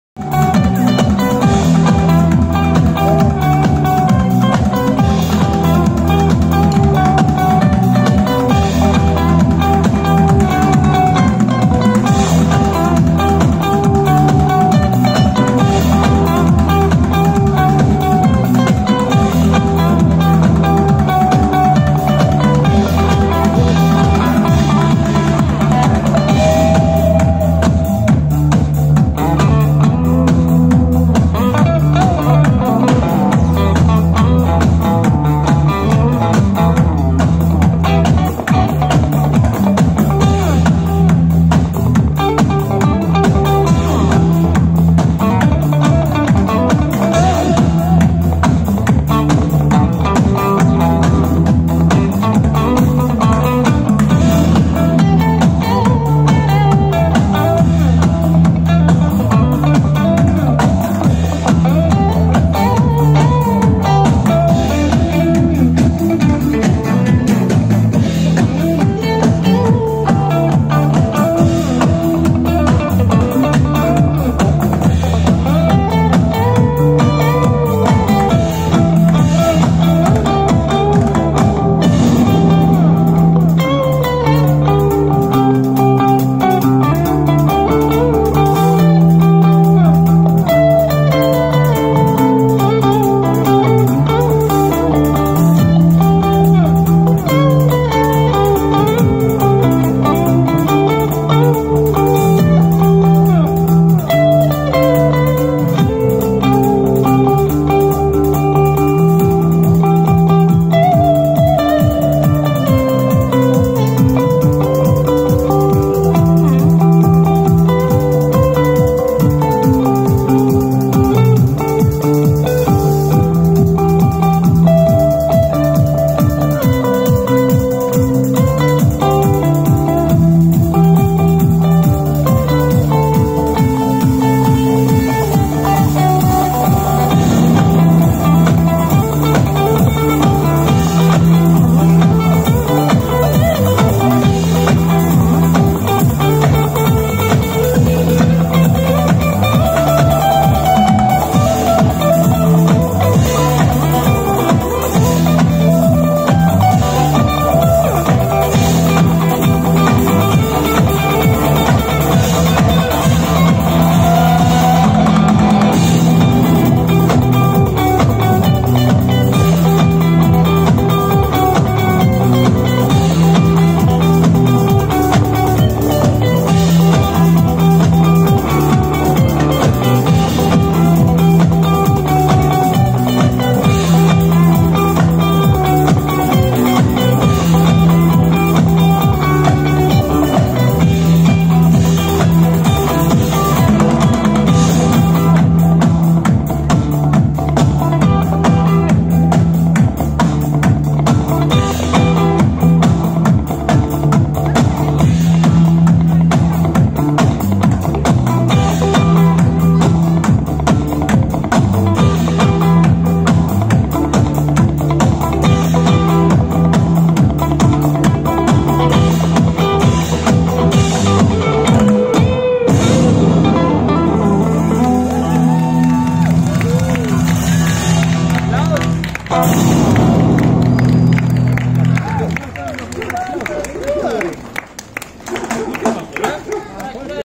Rocking musical performance by Level Spaces | Best street musicians
A rocking street musical performance by Level Spaces. I spotted them in Gesundbrunnen Center in Berlin.